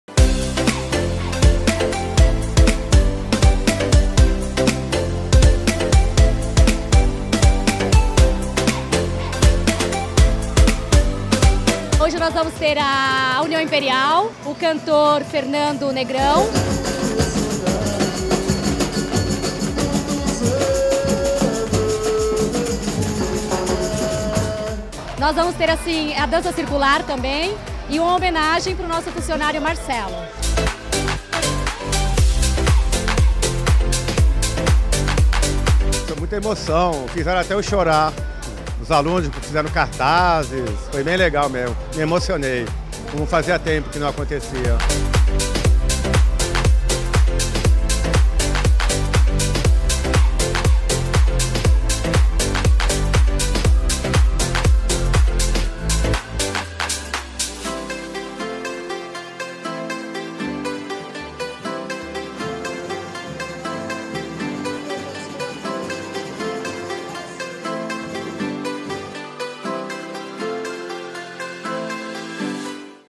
UME 'Emília Maria Reis' comemora 17 anos de atividades
Os 600 alunos da escola Emília Maria Reis, na Vila Belmiro, comemoraram nesta quarta (14) os 17 anos da unidade com bolo de chocolate, pipoca, vídeo e apresentação da banda do colégio. A novidade foi o anúncio da elaboração do hino da instituição.